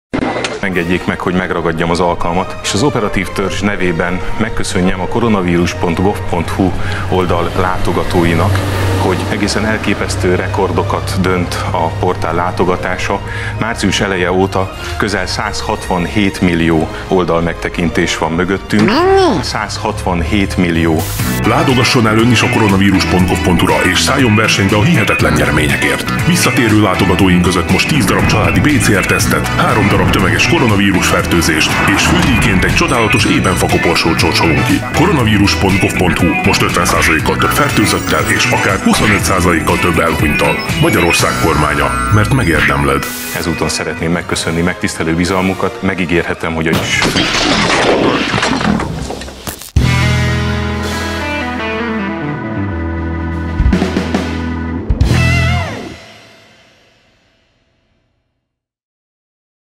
Gál Kristóf rendőrségi szóvivő úgy szállt be a marketingbe, mint egyszeri Vágási Feri az internetbe!
Gál Kristóf botcsinálta színészből lett botcsinálta szóvivő, de úgy tűnik, újabban már a reklámszakma felé kacsintgat. Segítünk neki!